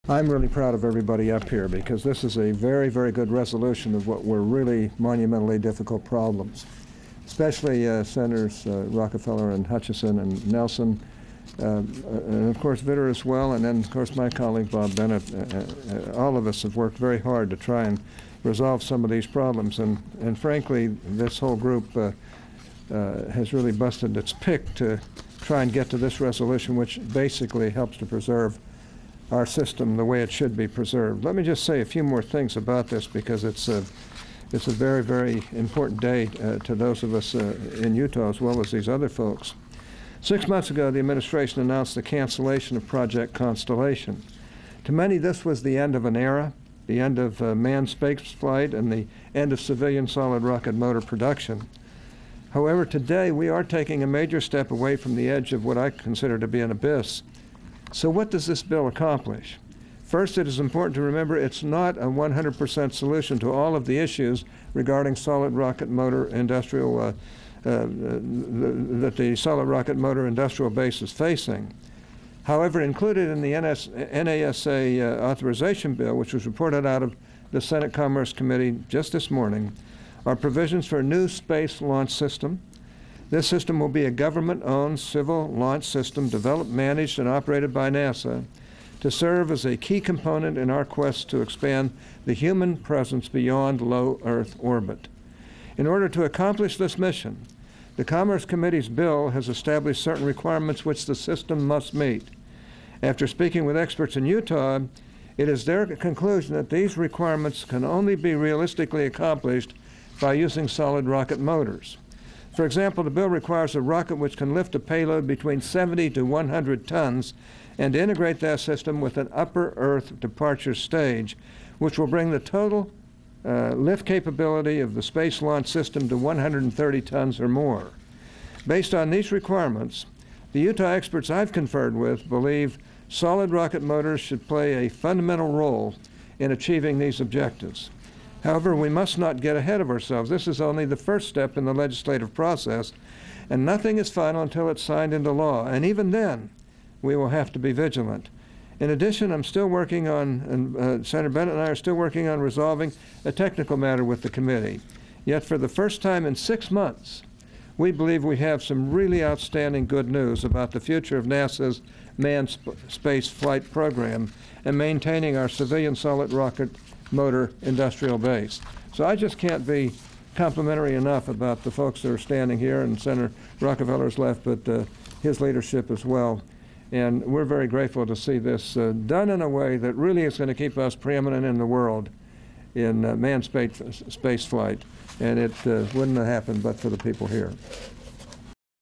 NASA Press Conference
Senator Hatch hails the NASA Authorization Bill as a possible "turning point" for the future of the nation's manned space flight program and the solid rocket motor industry in Utah.